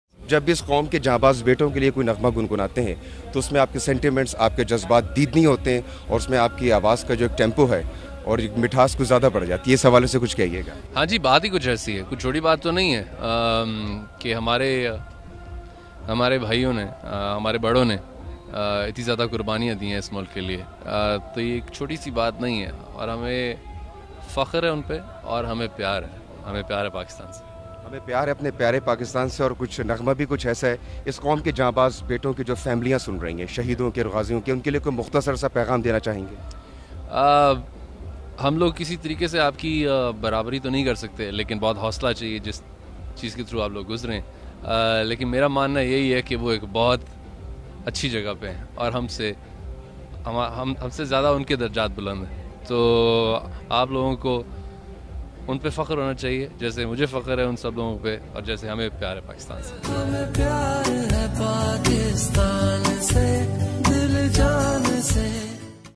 Atif Aslam & Athar Rizvi In Conversation
Video Snippet Was Recorded During The 6th, September Defense Day Event Held in GHQ Rawalpindi.